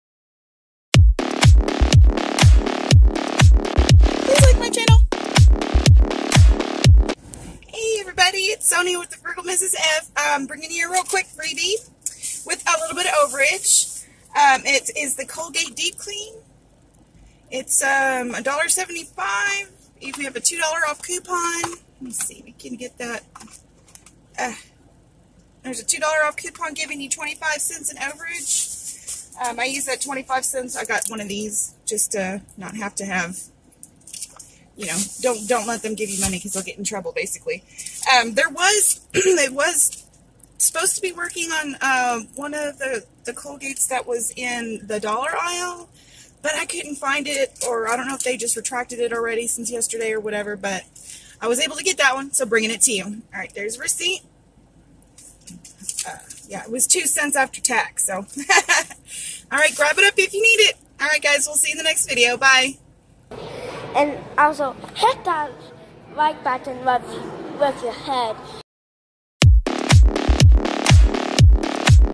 Freebie w/ Overage @ Dollar General!
Real quick video on what and how to do this deal for yourself!
I'm on Facebook too if you want to visit me there also. :)